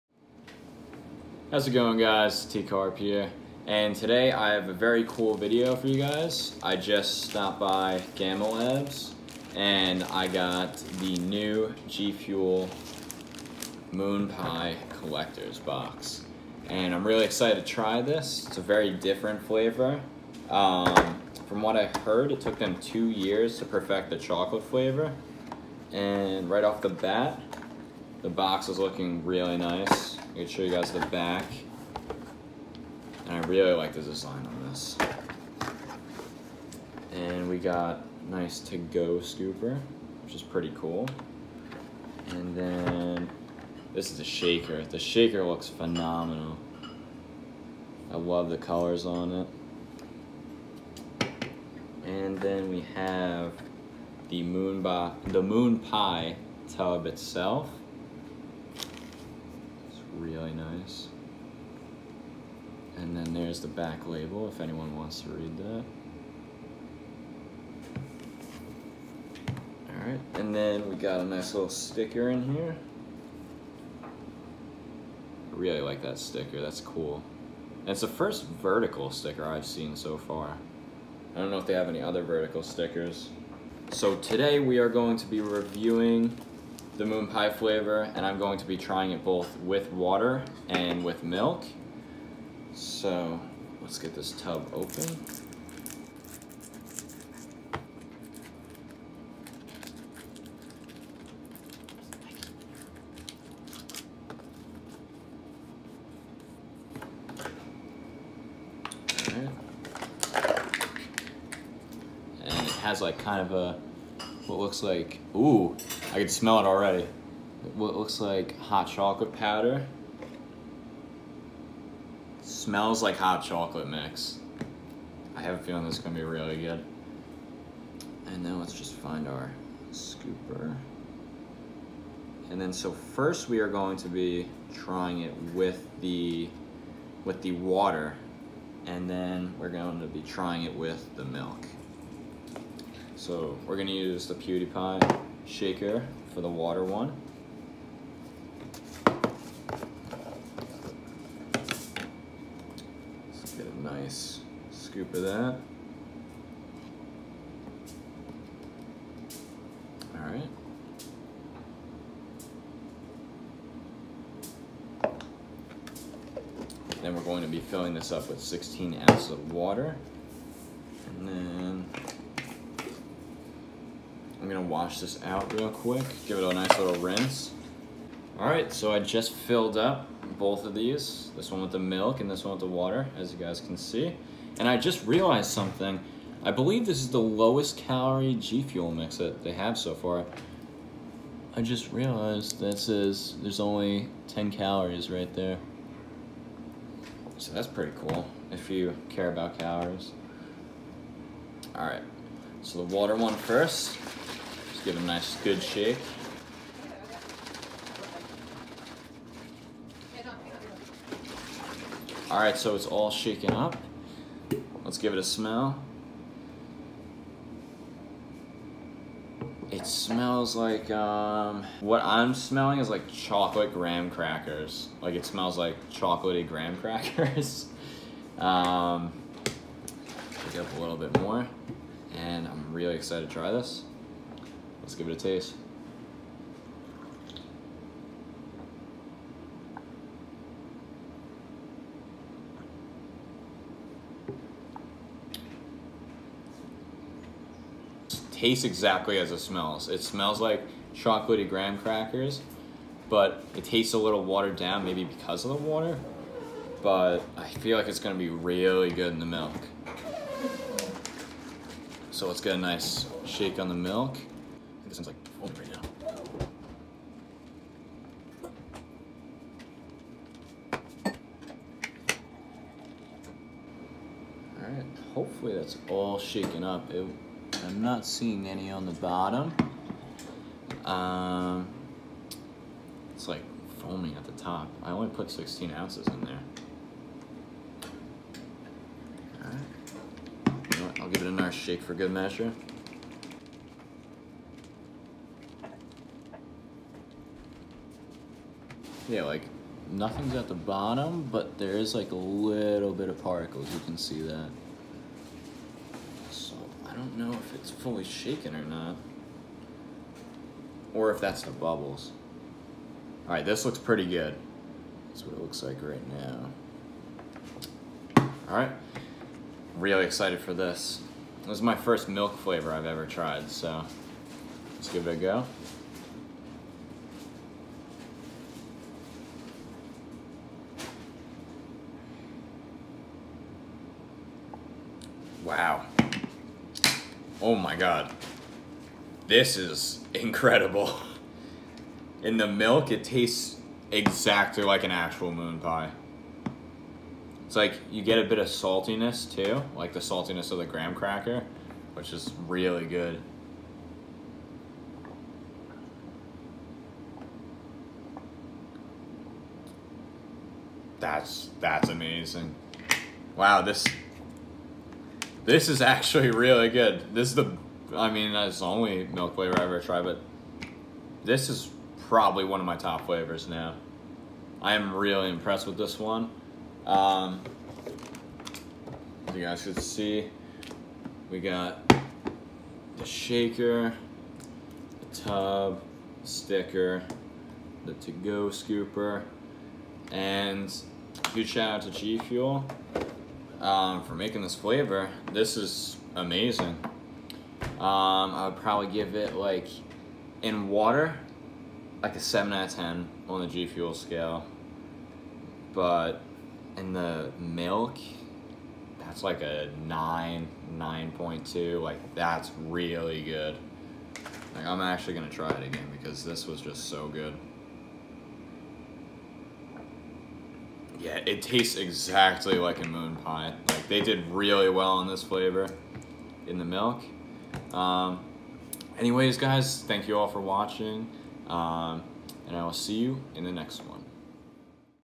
New GFUEL MOON PIE Flavor Review!!
I got this flavor day one of its release, and I wanted to share my first impressions of the flavor with you all! :)